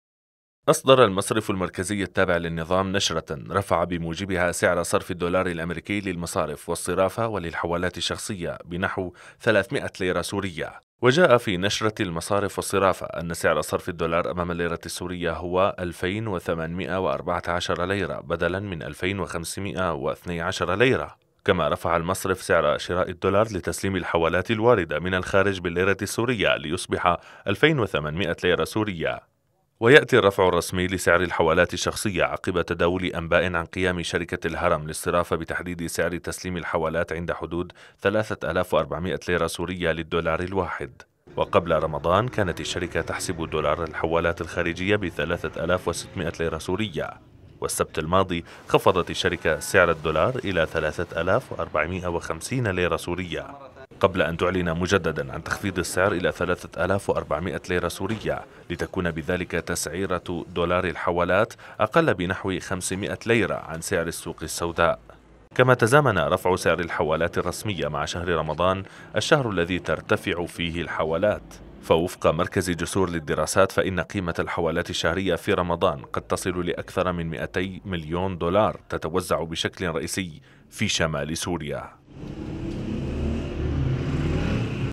المركزي يخفض سعر الليرة السورية أمام الدولار
تقرير: عبادة حيدري